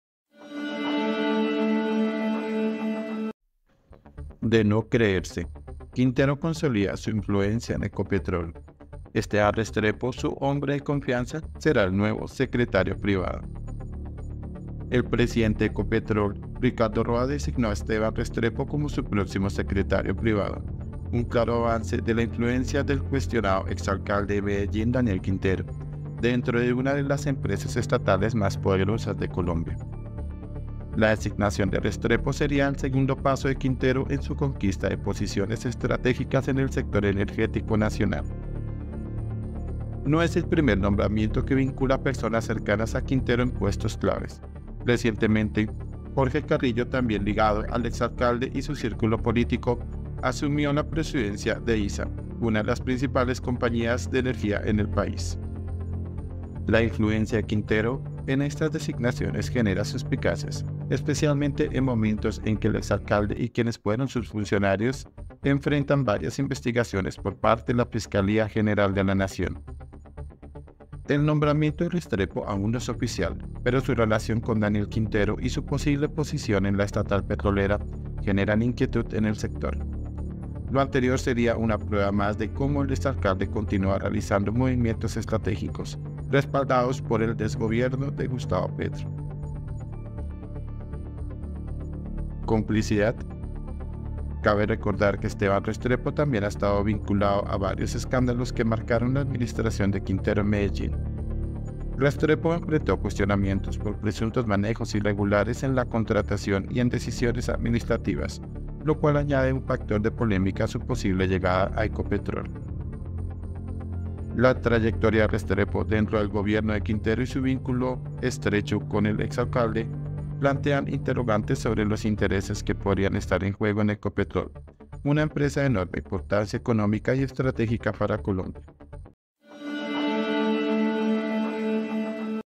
DE NO CREERSE 🔵 QUINTERO CONSOLIDA SU INFLUENCIA EN ECOPETROL
El presidente de Ecopetrol, Ricardo Roa, designó a Esteban Restrepo como su próximo secretario privado, un claro avance de la influencia del cuestionado exalcalde de  Medellín, Daniel Quintero, dentro de una de las empresas estatales más poderosas de Colombia.
IFM NOTICIAS